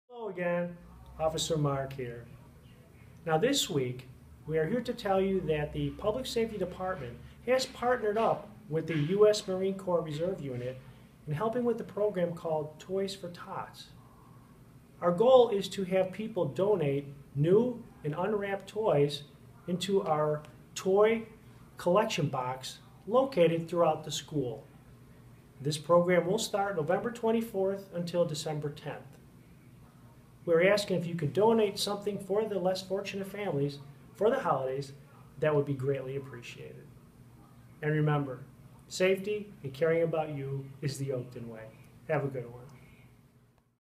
Toys for Tots Collection
Oakton Community College's Public Safety Department is teaming up with the U.S. Marine Corps Reserve Unit to collect new toys for the less fortunate November 24 - December 10.